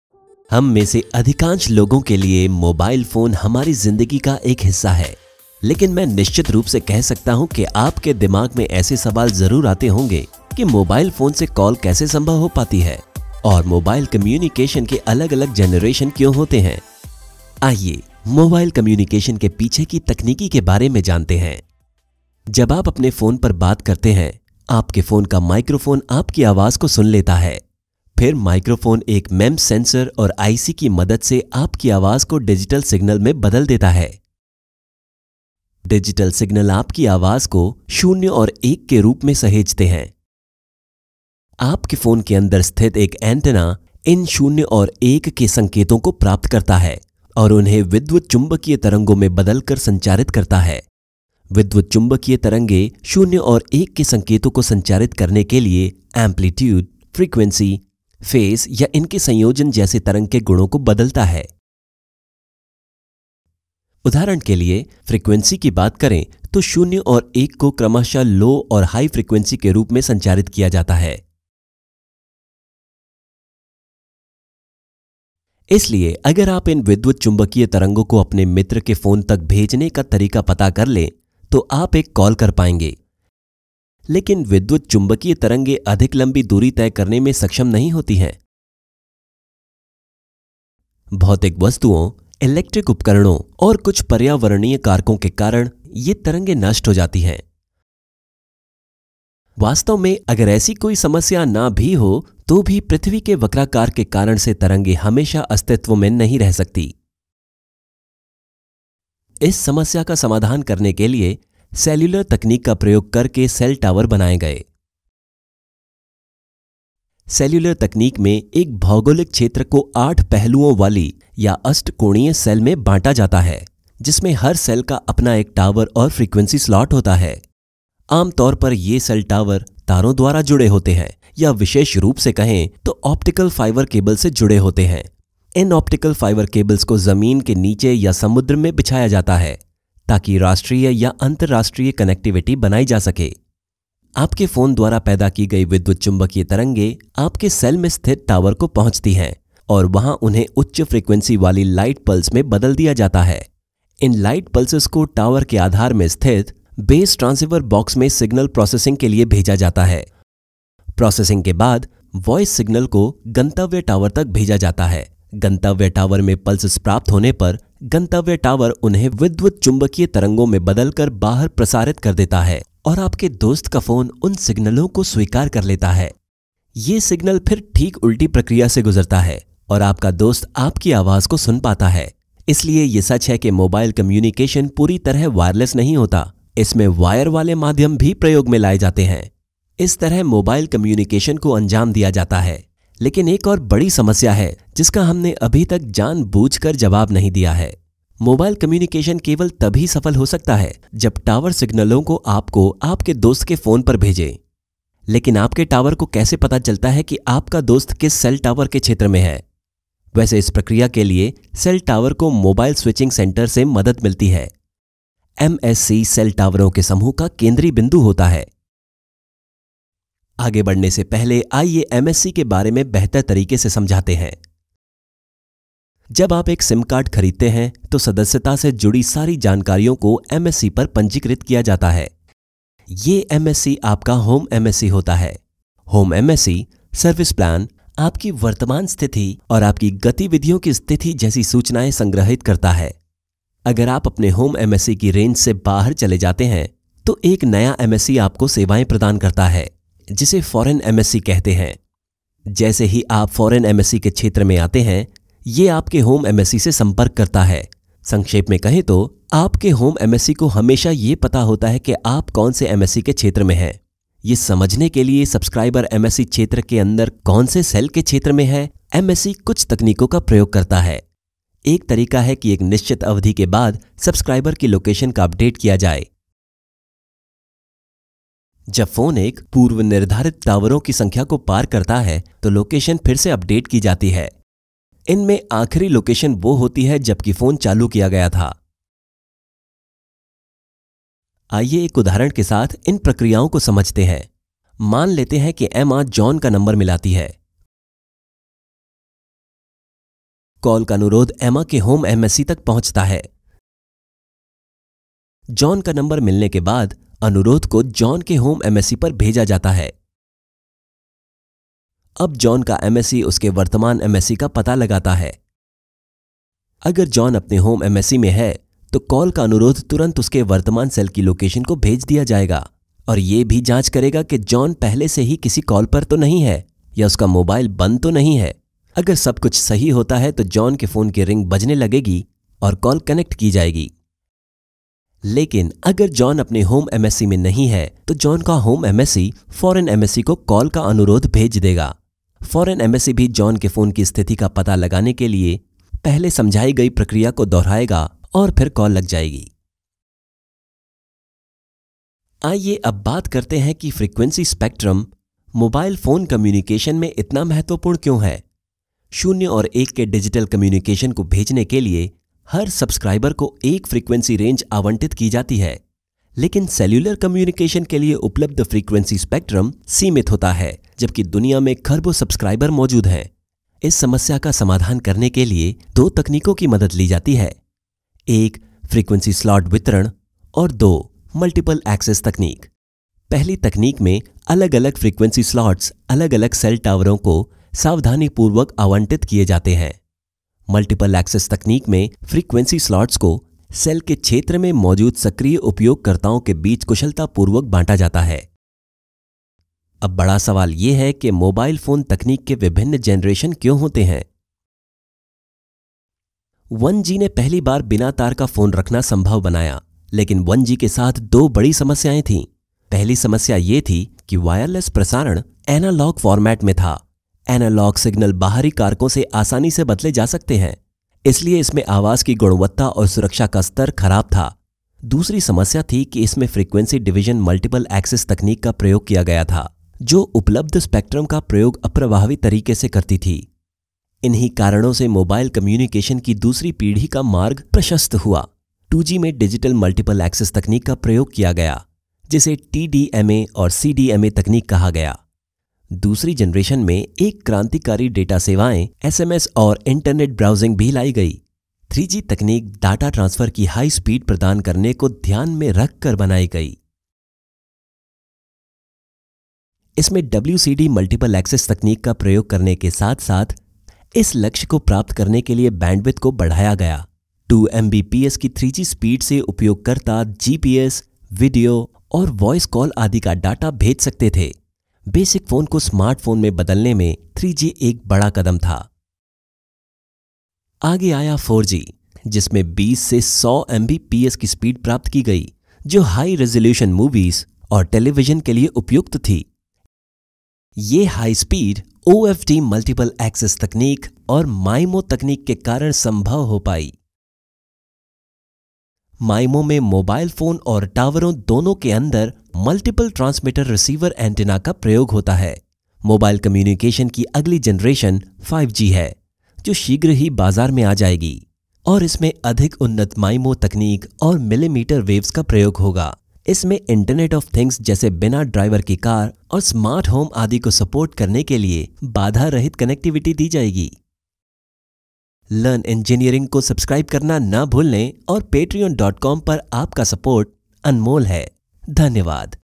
आपका मोबाइल फ़ोन कैसे काम करता है?
हम में से अधिकांश लोगों के लिए, मोबाइल फ़ोन हमारी ज़िन्दगी का एक हिस्सा हैं, लेकिन मैं निश्चित रूप से कह सकता हूँ कि आपके दिमाग में ऐसे सवाल ज़रूर आते होंगे कि मोबाइल फोन से कॉल कैसे संभव हो पाती है और मोबाइल कम्युनिकेशन के अलग-अलग जेनरेशन क्यों होते हैं| आइए मोबाइल कम्युनिकेशन के पीछे की तकनीकी के बारे में जानते हैं|